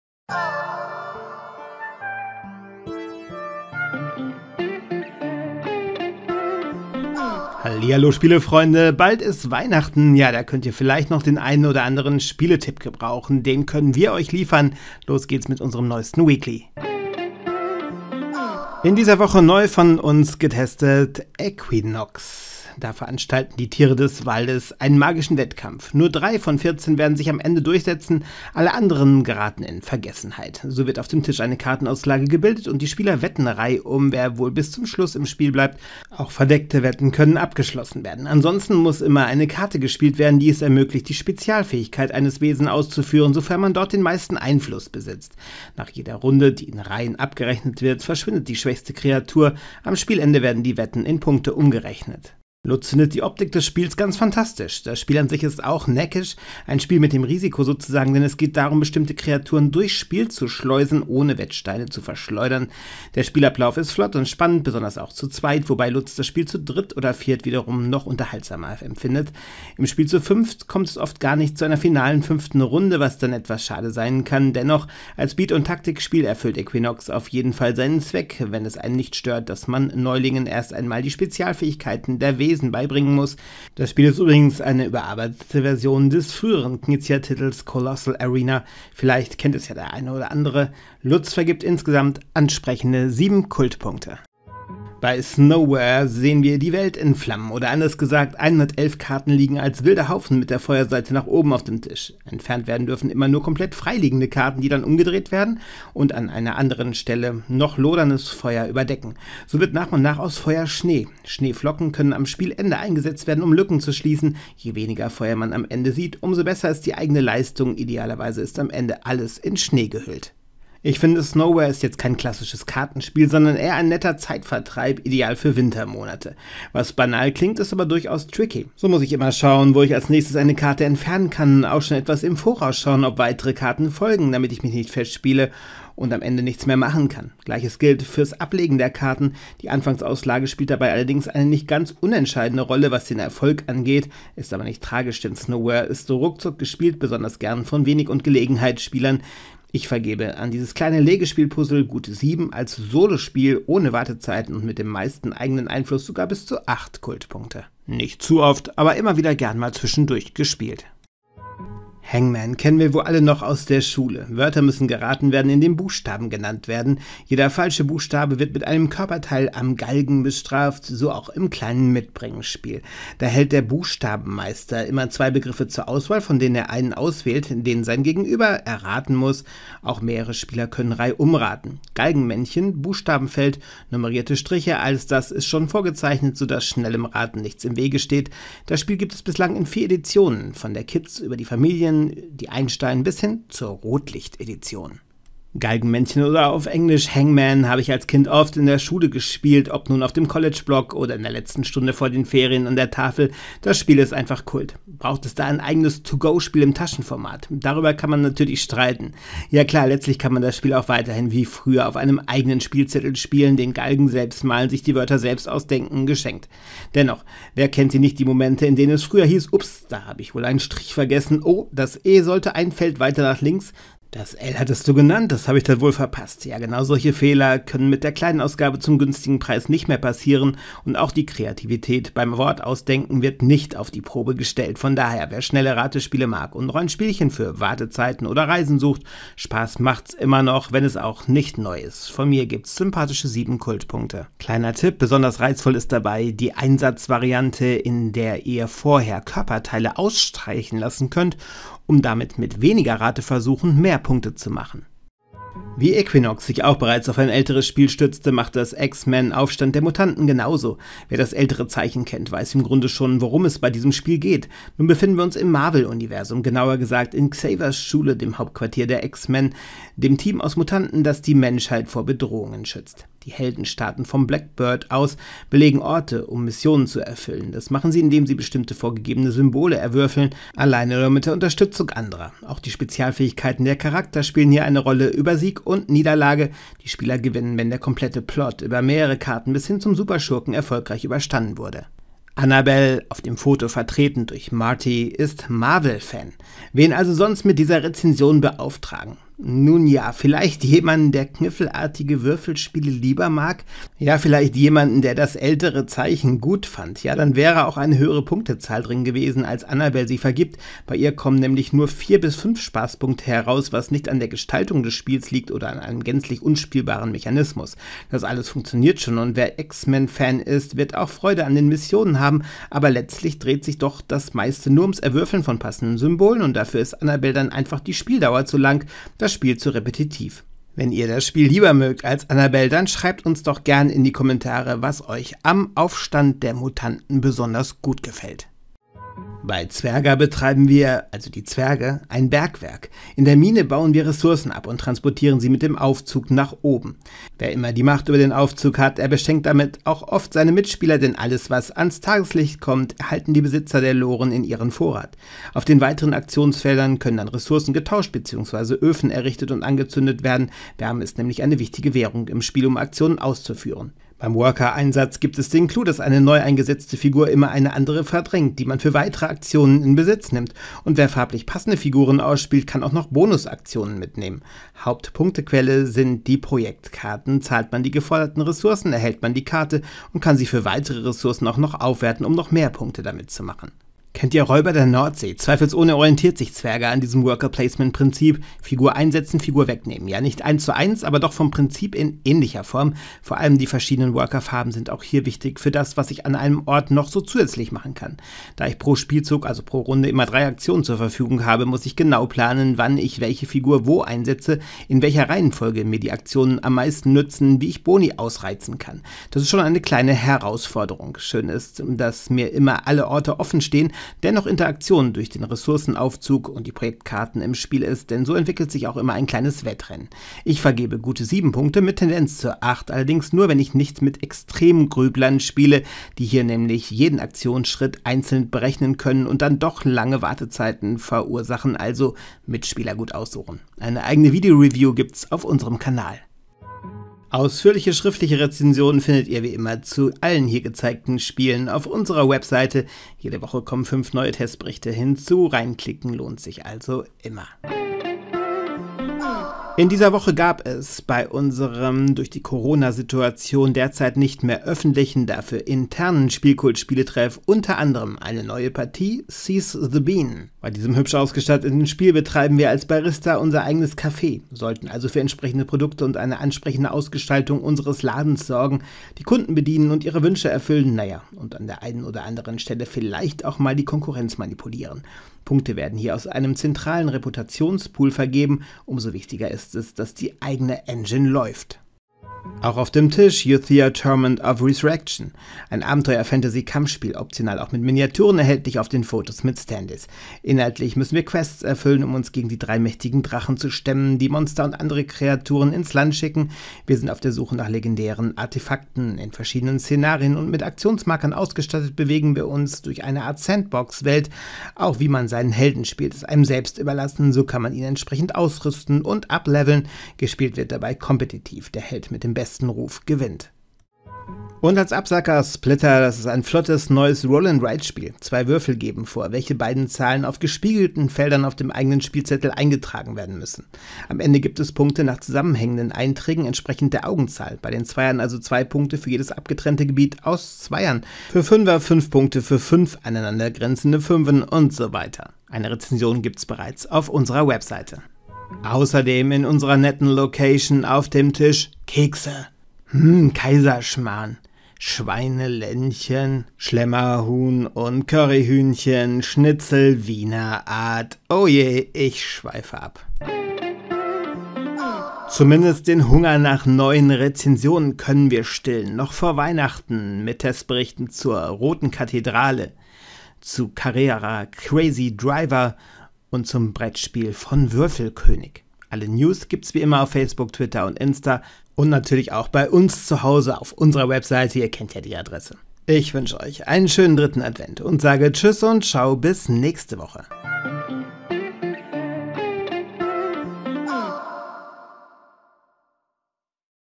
Weekly 49/2021 // Brettspiel-Reviews, Spieletreff, Preview
Das SPIELKULT-Wochenmagazin.